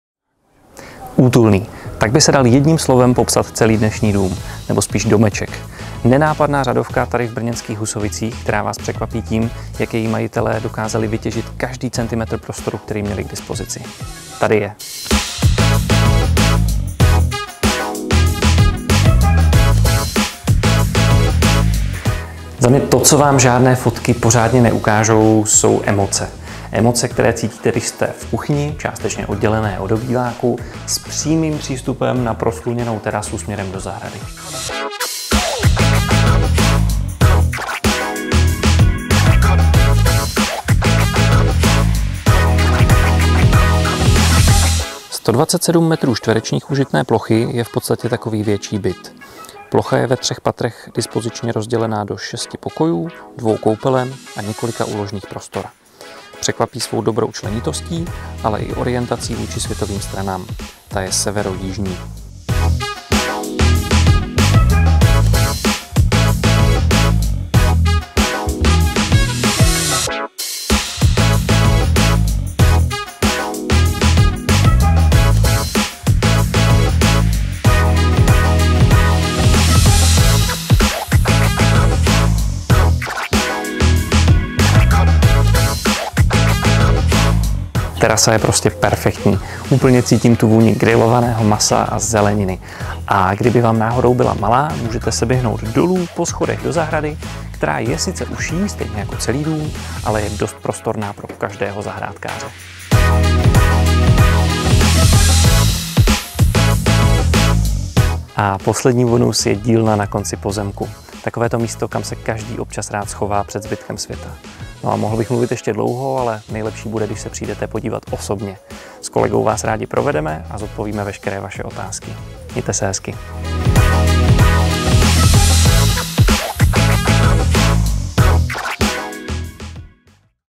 Prodej rodinného domu, 134 m2, pozemek 259 m2, Hálkova, Husovice, Brno
Videoprohlídka k prodeji útulného rodinného domu v brněnských Husovicích o užitné ploše 134 m2 a s krásnou zahradou.